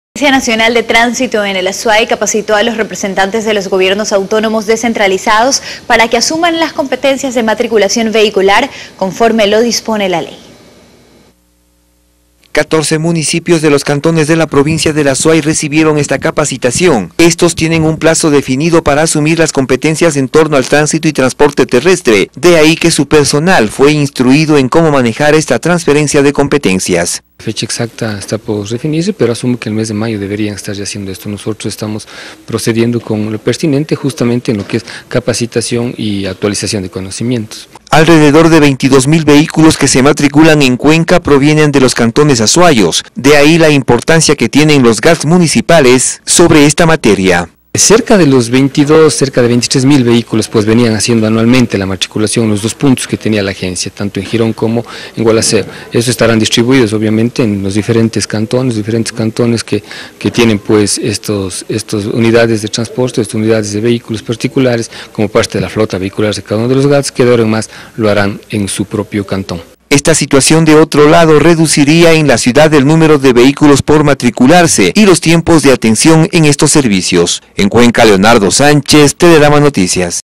ANT capacita a GAD'S para asumir competencias en tránsito
La agencia nacional de tránsito en el Azuay, capacitó, a los representantes de los gobiernos autónomos descentralizados para que asuman las competencias de matriculación vehicular conforme lo dispone la ley.